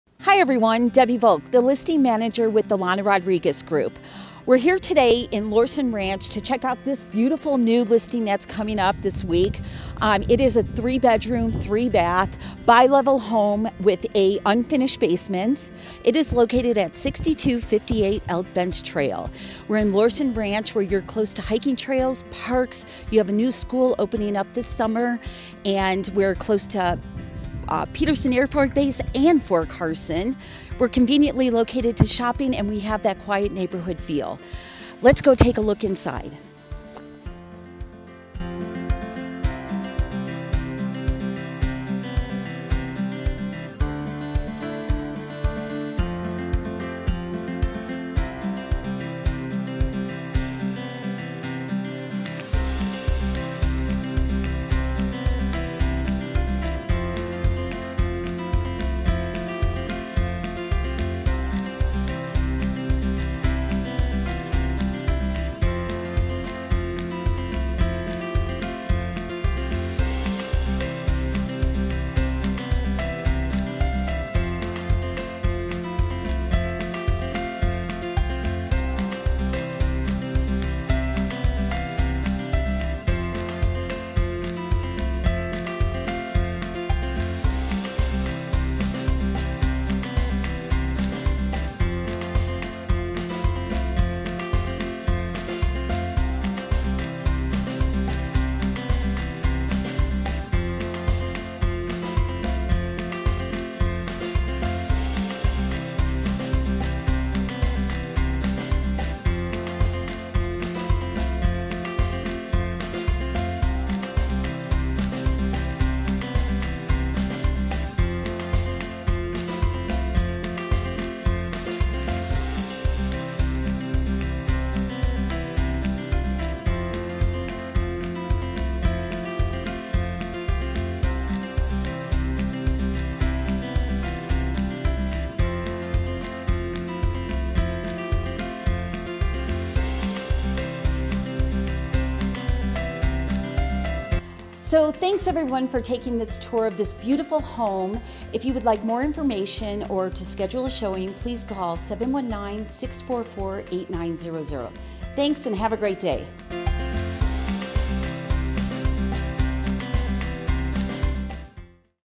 The Lana Rodriguez Group - 6258 Elk Bench Trail, Colorado Springs, CO
Look no further! You will love this Beautiful home in Lorson Ranch. This home has 3 bdrms, 2.5 baths, 3 Car Tandem Garage and features Gorgeous Solid Oak Hardwood floors on the Main level and Lower level.The Fabulous Kitchen has vaulted ceilings, all Stainless steel appliances (included refrigerator), Breakfast Bar and Extra Deep Composite Granite sink. The floor plan allows for great entertaining with the open feeling of the kitchen, dining and family room. Upstairs is the Master Suite complete with Ceiling fan and 5 Piece Master bath, with double sinks and a separate shower. The upper level also has the conveniently located Over-sized Laundry room, additional full bath and 2nd & 3rd bedrooms. Stay cool with Air Conditioning plus Humidifier! Garage has a service door that leads to a Big Fenced Yard with a patio and Auto Sprinkler System. This home is Conveniently located near Peterson AFB, Fort Carson and Shriever AFB.